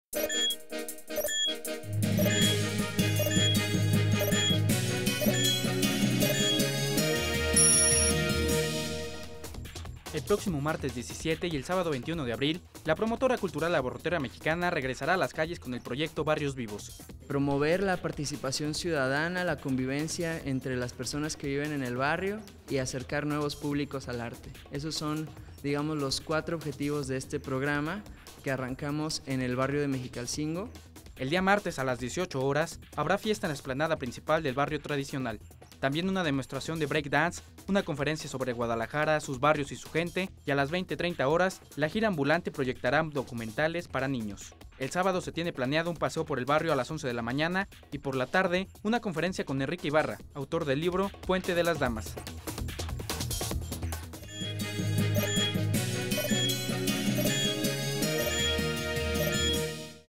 Corte 44 - "Barrios Vivos" Regresa a la calle - 17 de Abril de 2012
El próximo martes 17 y el sábado 21 de abril, la Promotora Cultural Abarrotera Mexicana regresará a las calles con el proyecto "Barrios vivos".
El día martes a las a las 18:00 horas, habrá fiesta en la explanada principal del barrio tradicional, también una demostración de Brey Dance, una conferencia sobre Guadalajara "Sus barrios y su gente" y a las 20:30 horas la gira ambulante proyectará documentales para niños.

El sábado se tiene planeado un paseo por el barrio a las 11:00 de la mañana y por la tarde una conferencia con Enrique Ibarra,  autor del libro "Puente de las damas".